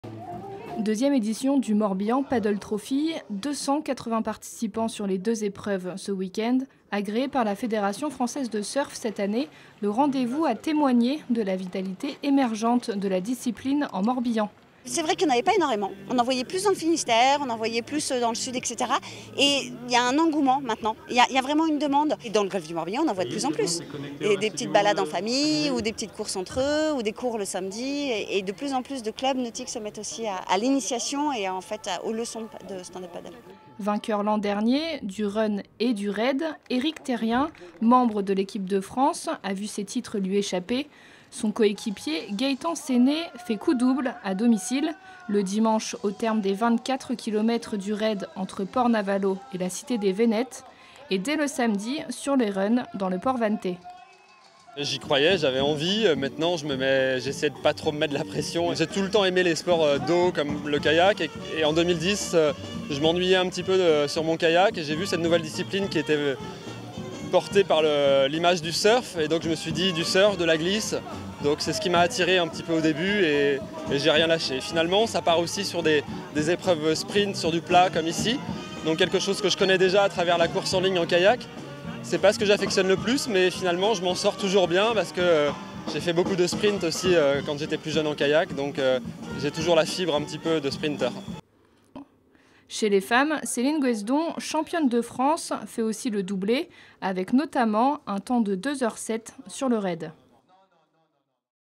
2e Morbihan Paddle Trophy : Séné à la maison
280 participants prenaient part à cette 2ème édition du Morbihan Paddle Trophy, agréée cette année par la Fédération française de surf (l'épreuve comptait pour les classements nationaux de Sup Race). La discipline émerge petit à petit dans le Morbihan.
Vainqueur l'an dernier, le membre de l’Équipe de France Eric Terrien a vu son titre lui échapper au profit de son coéquipier Gaëtan Séné. Le Morbihannais remporte le run en 6min33 et s'impose également dimanche sur les 24 km du raid entre Port-Navalo et Vannes en 1h49. Chez les femmes, Céline Guesdon, championne de France s'impose également sur le run et sur le raid (2h07).

Reportage de Clara Jestin pour la rédaction des sports de TébéSud (images drone de GFmédias)